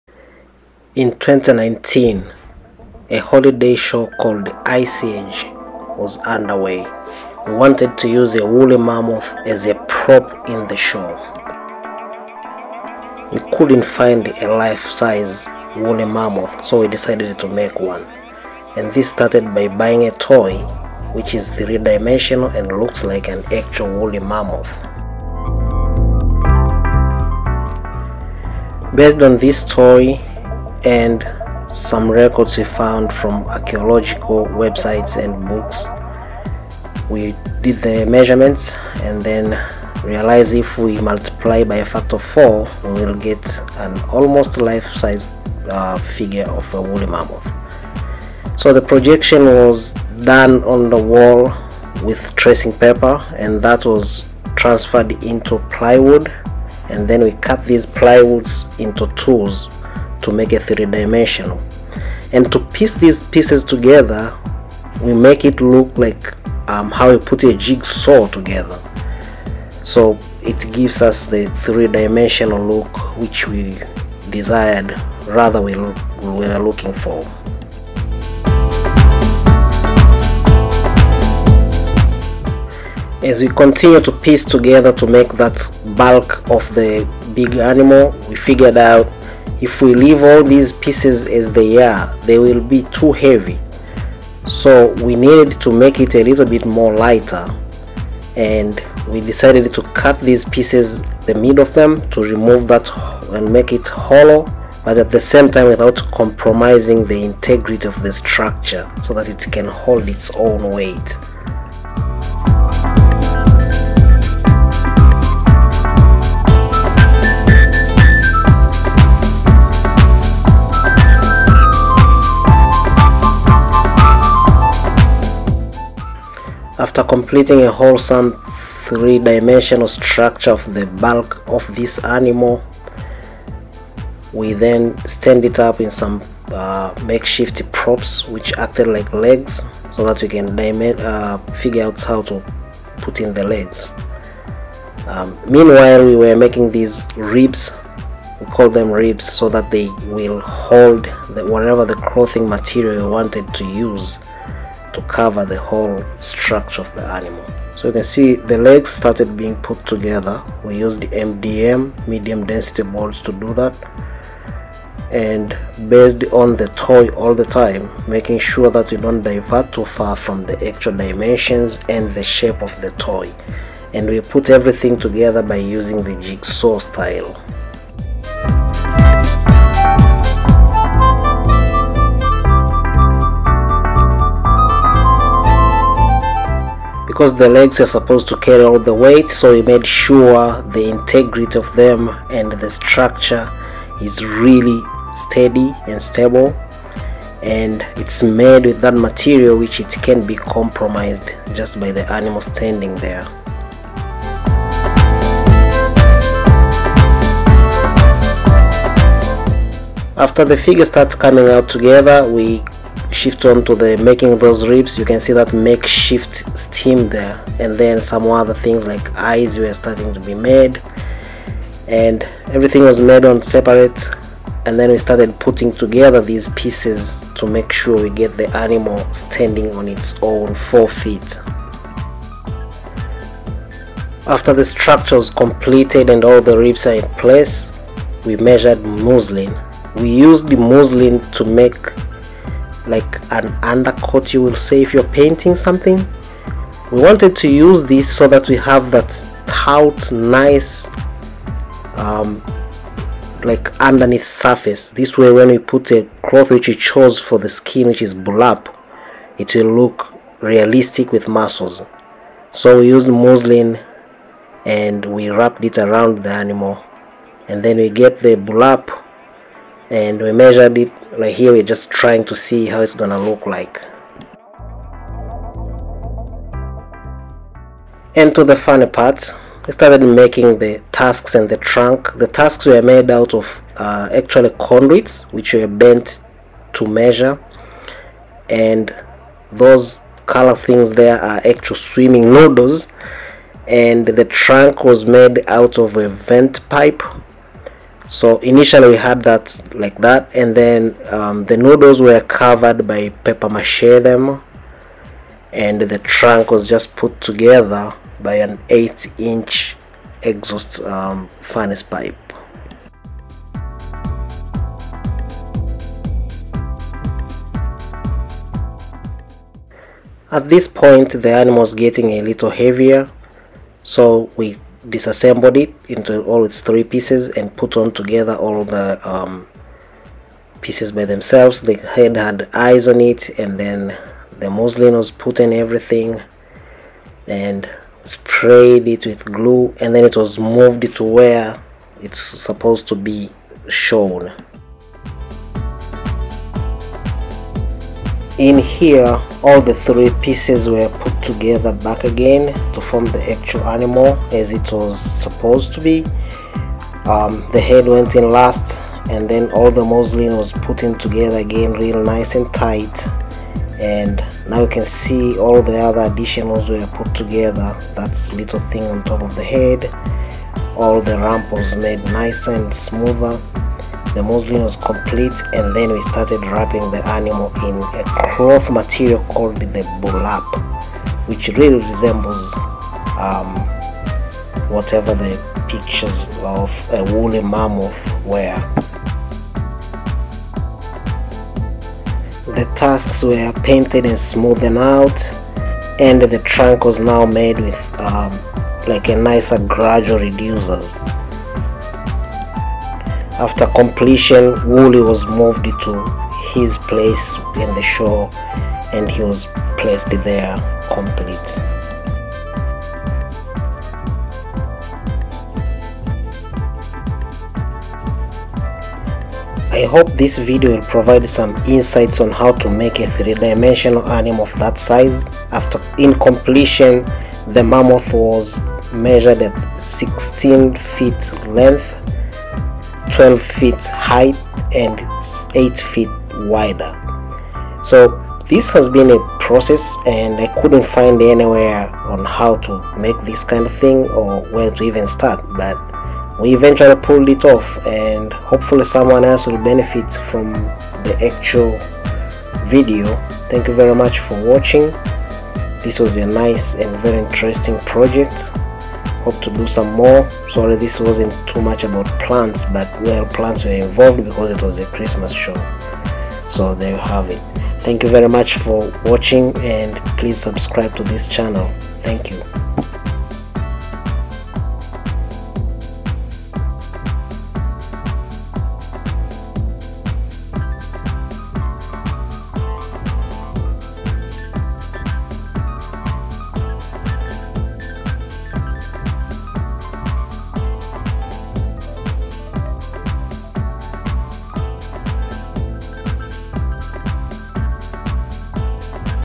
The making of A Woolly Mammoth | Mammuthus primigenius | Ice Age | Holiday Display
For a Christmas show titled ICE AGE we figured the best prop to accompany all the icy backdrops and white poinsettias we will use an almost life size woolly mammoth. We couldn't find this anywhere nor could we find tutorials on how to make one so we we bought a toy and projected it into tracing paper the desired dimensions and then use .25" plywood to make a structure,then we wrapped muslin around it and cover it with "burlap". the results were astonishingly realistic and here is a video of pictures I took through the process.